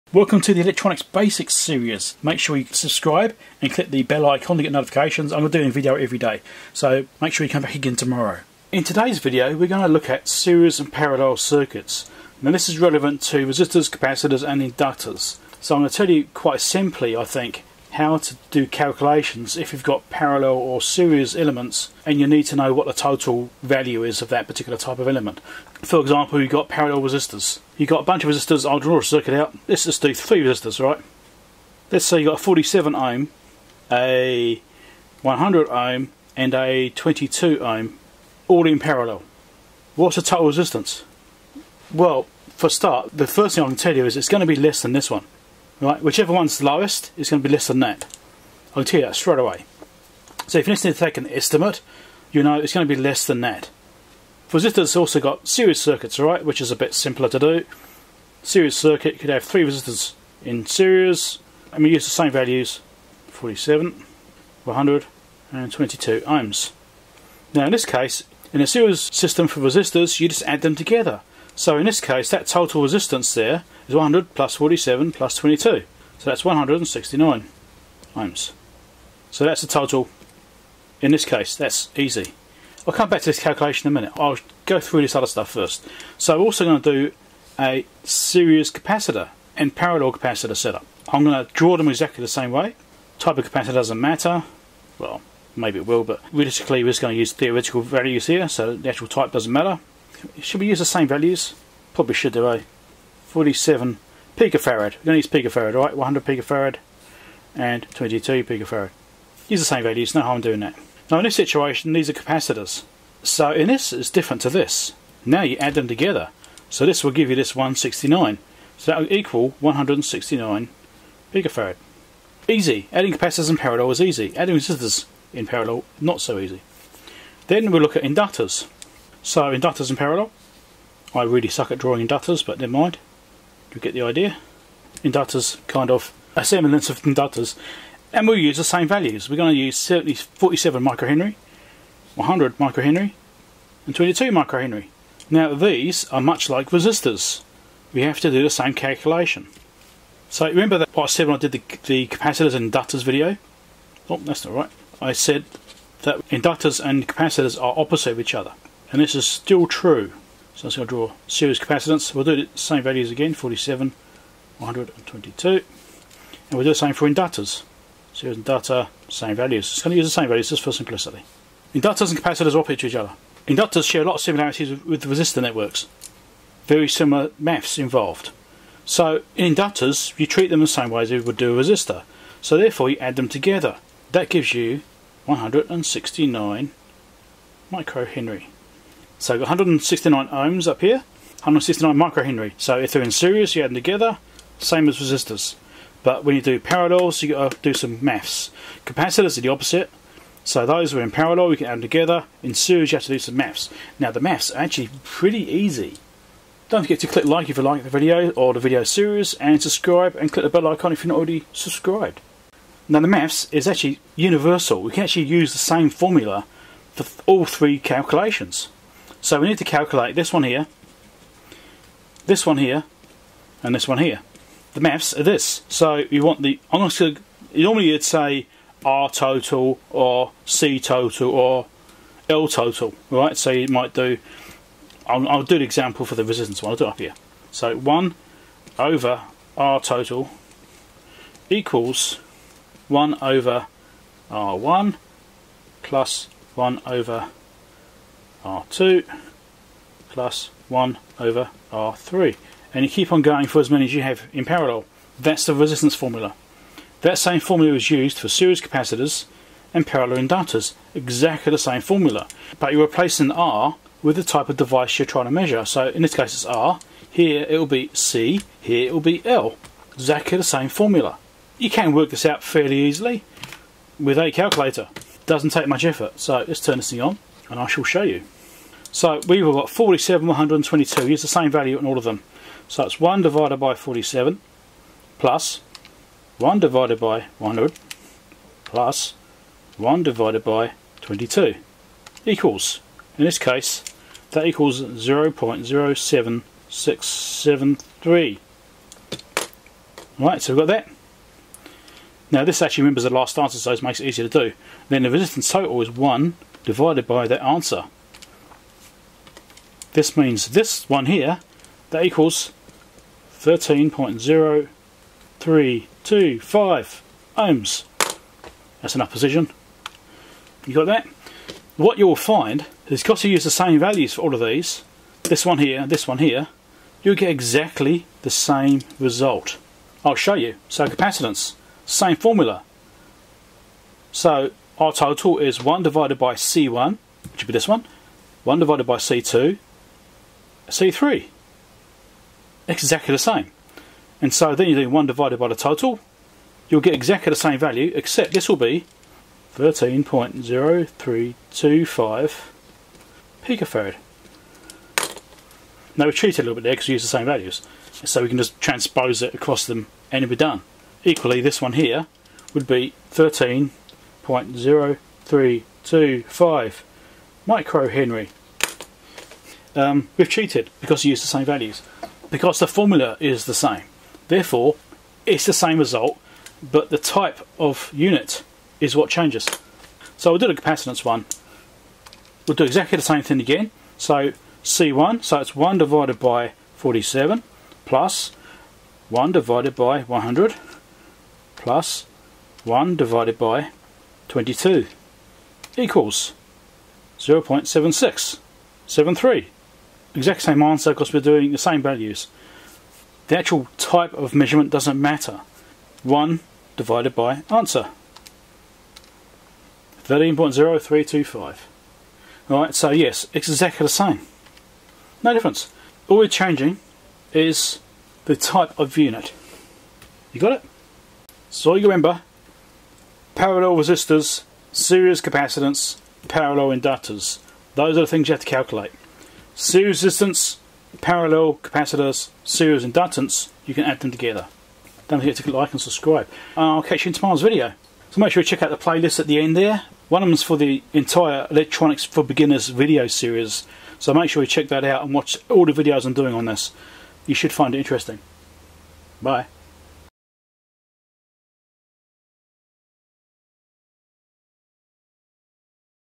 🔴 Electronics For Beginners - No.14 - Series & Parallel Resistors, Capacitors, Inductors - No.977
Electronics For Beginners - No.14 - Calculating Series and Parallel Resistors, Capacitors, Inductors

The video series where I teach you about electronics, for newbies and hobbyists.

My Electronics Tools (video and purchase links)
Boonton
4220.

HP/Agilent/Keysight
3457A.

Marconi
2022D.

Siglent
SDS1104X-E.
SPL-1008.